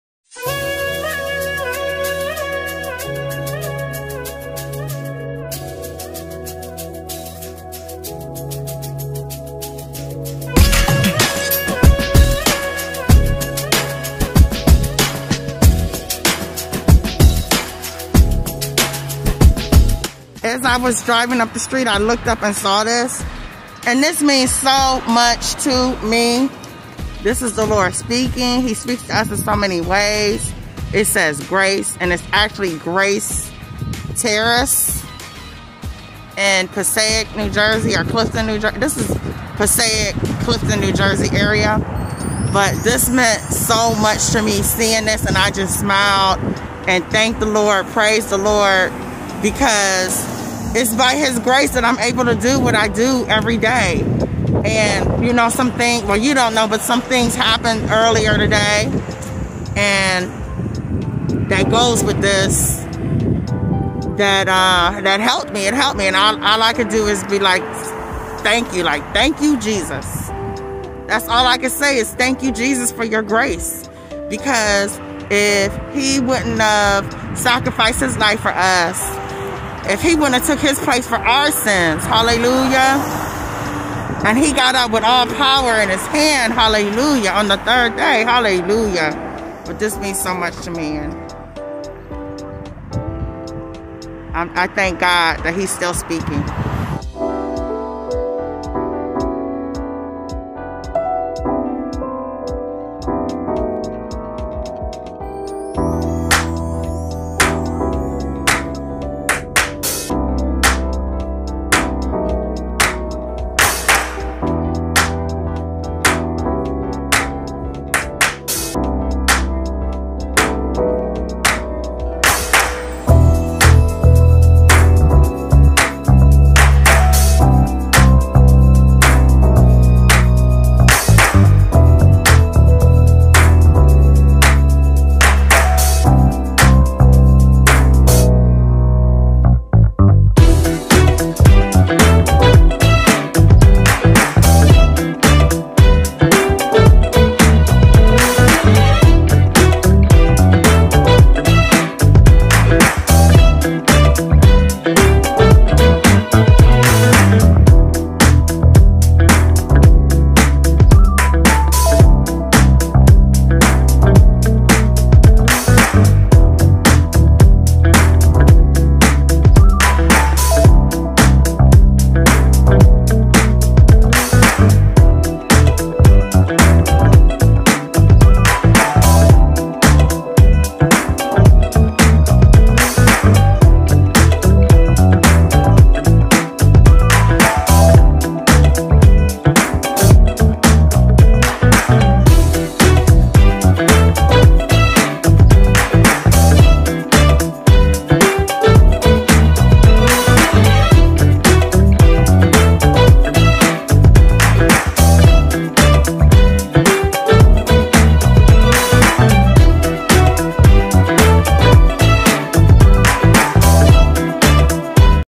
GRACE TESTIMONY, A CONVERSATION
BLESSINGS IN THE NAME OF THE MOST HIGH LORD!! THANK YOU FOR STOPPING BY!! I APPRECIATE YOU!! ONE DAY I WAS GOING THROUGH AND AS I WAS DRIVING I SAW A STREET SIGN THAT SAID GRACE. IN THE SPIRIT THE MOST HIGH LORD WAS SPEAKING TO ME. TO THE GLORY OF GOD IN JESUS CHRIST OF NAZARETH NAME I'M SHARING THIS TESTIMONY, TRANSPARENCY WITH EVERYONE.
BE ENCOURAGED &  BLESSED❤️
📖⚔️Ephesians 3:20-21
(AMP)📖⚔️
20 Now to Him who is able to [carry out
 His purpose and] do superabundantly
 more than all that
we dare ask or
think [infinitely beyond our greatest prayers, hopes, or dreams],
according to His power that is at work within us,
21 to Him be the glory in the church and in
Christ Jesus throughout
all generations
forever and ever. Amen.